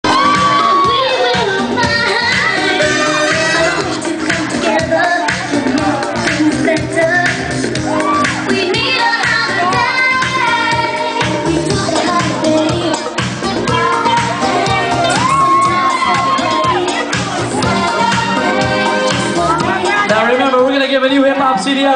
dj gudah bloopers
a guest goes a little too low and hits the floor pretty hard.